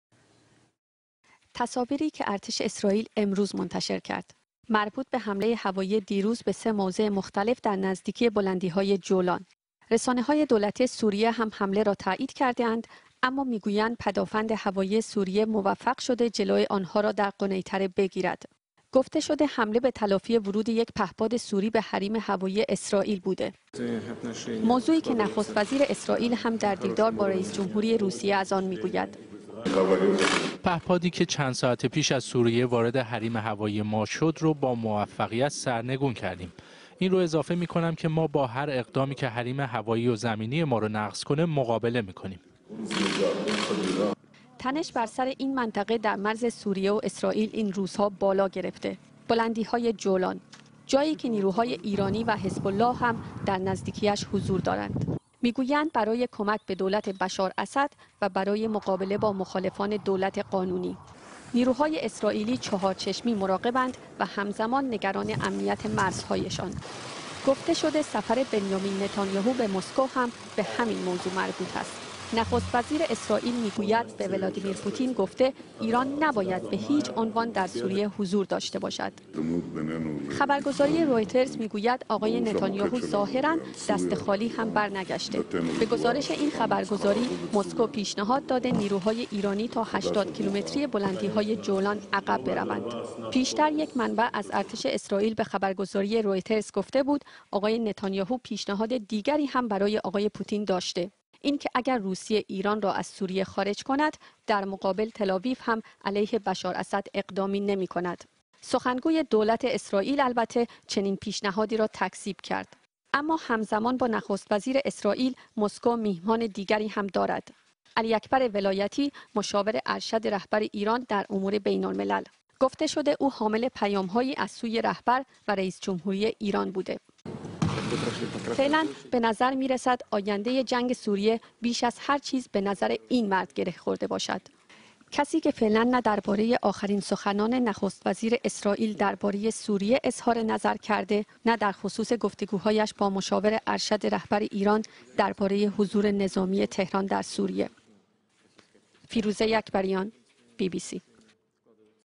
Israeli Premier in Moscow talk bout bombing Syria with V.Putin
Israeli Premier in Moscow talk bout bombing Syria with V.Putin Watch related video [Israel bombed T14 military base in Syria]